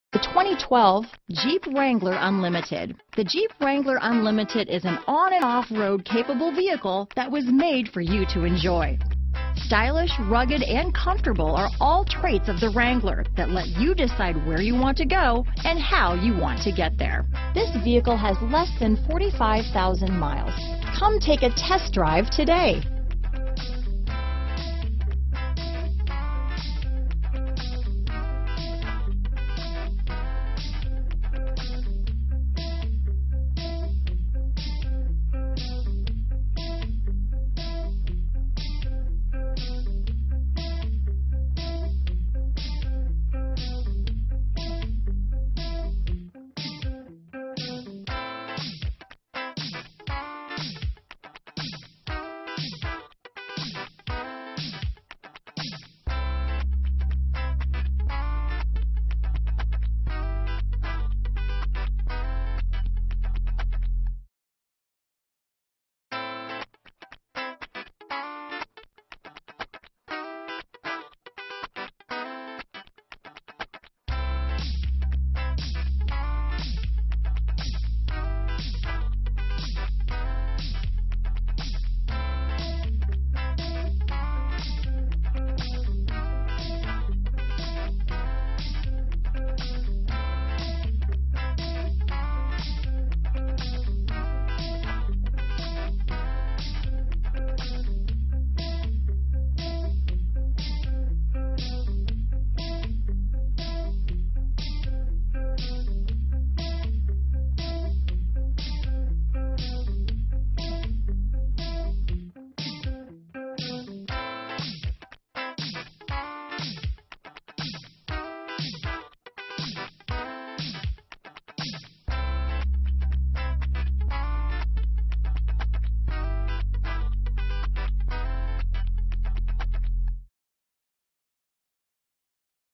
2012 JEEP WRANGLER UNLIMITED Santa Rosa, Petaluma, Marin, San Francisco, North Bay, CA CL222646
BRIGHT SILVER METALLIC CLEARCOAT USED 2012 JEEP WRANGLER UNLIMITED available in Santa Rosa, California at Lithia Chrysler Jeep Dodge of Santa Rosa. Servicing the Petaluma, Marin, San Francisco, North Bay, CA area.
2012 JEEP WRANGLER UNLIMITED 4WD 4DR - Stock#: CL222646 - VIN#: 1C4BJWDG1CL222646

2727 Dowd Dr.

Odometer is 24425 miles below market average! CARFAX One-Owner.  Bright Silver Metallic 2012 Jeep Wrangler Unlimited Sport 4WD 5-Speed Automatic 3.6L V6 24V VVT 16/20mpg   Awards:   * 2012 KBB.com Total Cost of Ownership Awards   * 2012 KBB.com Best Resale Value Awards